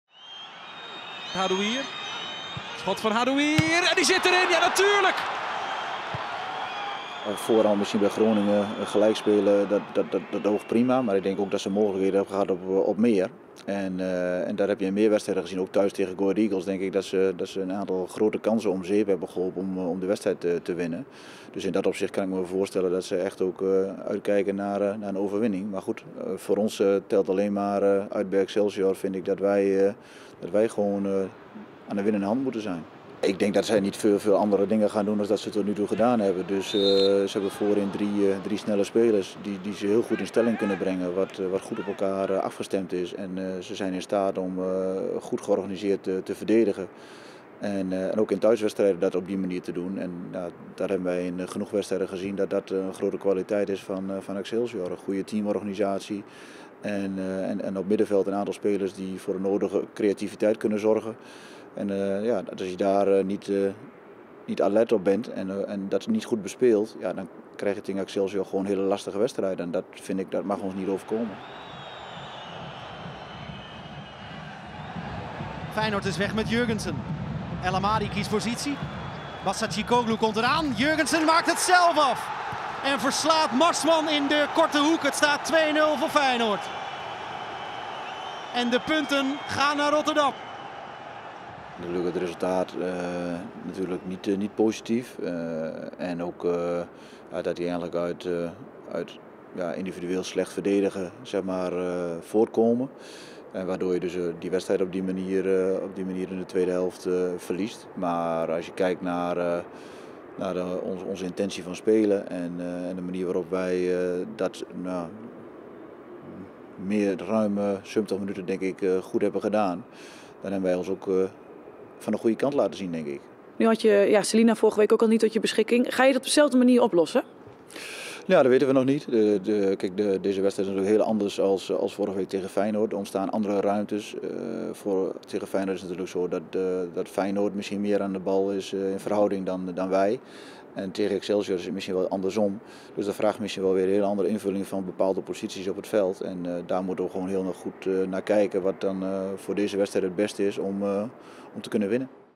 Op bezoek in Kralingen
Zondagmiddag neemt FC Twente het op tegen Excelsior. Die ploeg heeft een goed gevoel overgehouden aan de vorige speelronde toen er een gelijkspel werd gehaald in de uitwedstrijd tegen FC Groningen.